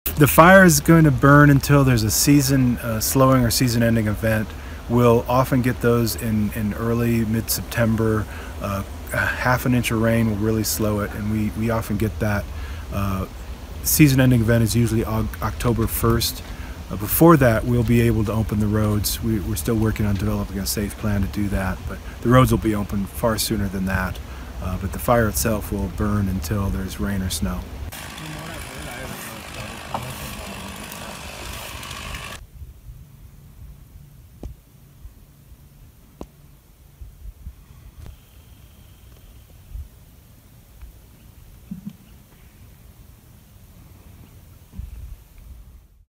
Firefighter: Teton Fire May Burn Into September
The wildfire inside Grand Teton National Park forced officials to close a road leading north to Yellowstone National Park. A fire incident commander says he expects the Grand Teton blaze to burn well into September. (Aug. 26)